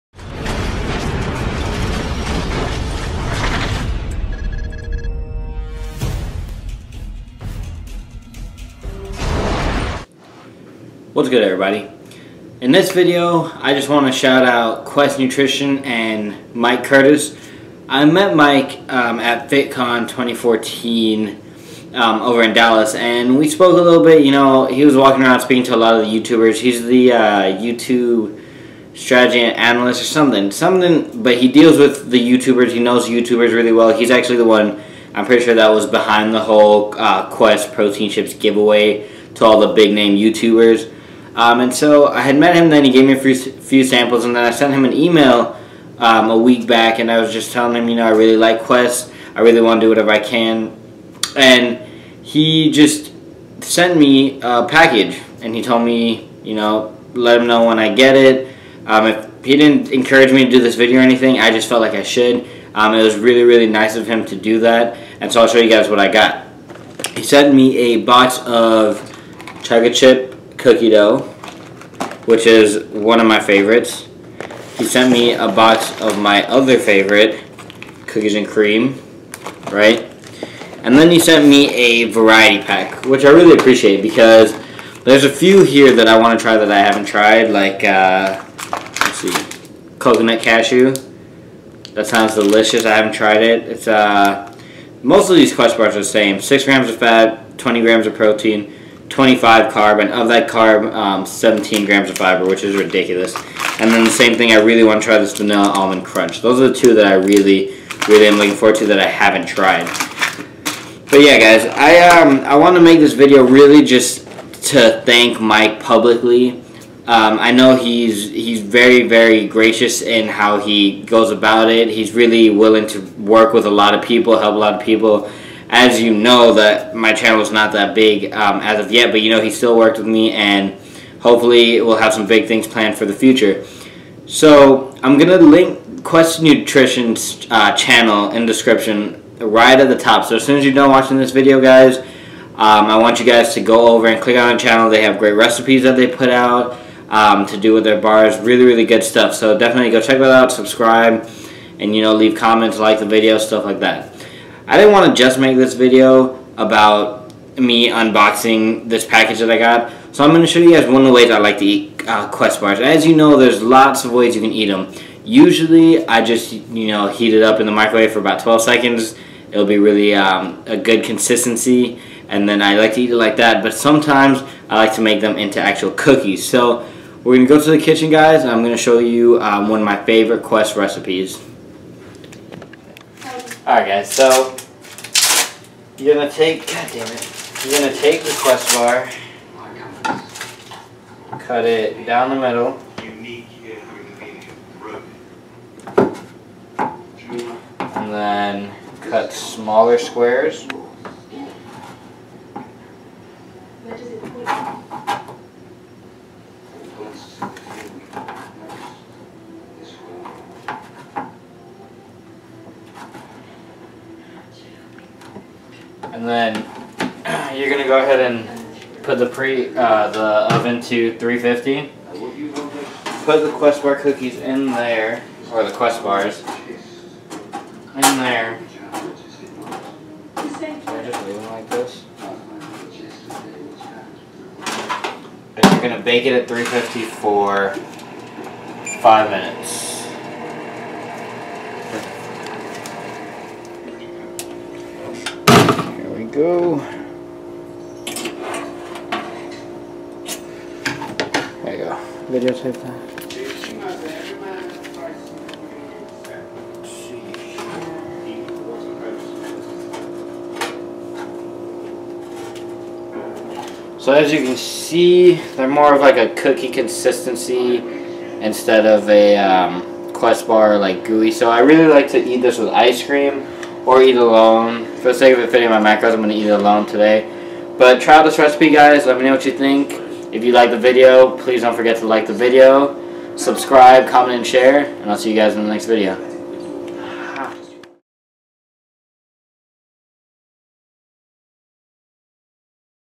Delicious 5 Minute Quest Bar Recipe!
This is a video I made to talk about Mike Curtis and show you guys a quick Quest Bar recipe I like to use. It only takes 5 minutes!